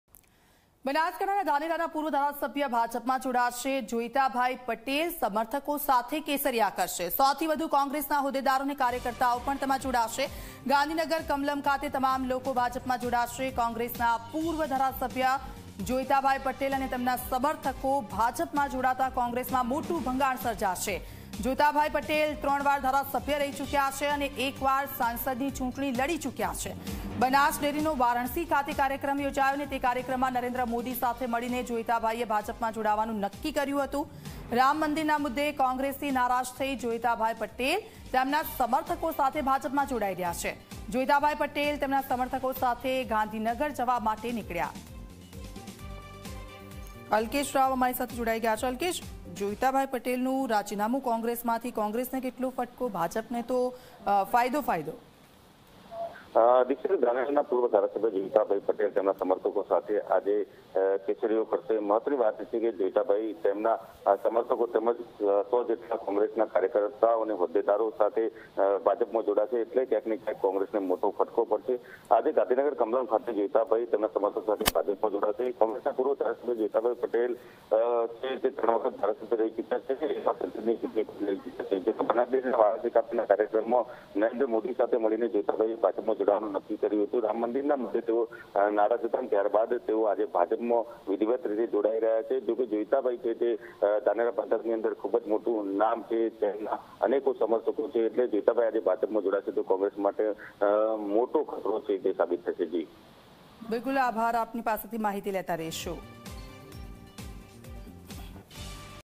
Banaskantha: ધાનેરાના પૂર્વ ધારાસભ્ય જોડાશે ભાજપમાં : જોઈતાભાઈ પટેલ સમર્થકો સાથે આજે કેસરિયા કરશે
બનાસકાંઠાના ધાનેરાના પૂર્વ ધારાસભ્ય જોડાશે ભાજપમાં : જોઈતાભાઈ પટેલ સમર્થકો સાથે આજે કેસરિયા કરશે

- 100થી વધુ કોંગ્રેસના હોદ્દેદારો અને કાર્યકર્તા પણ જોડાશે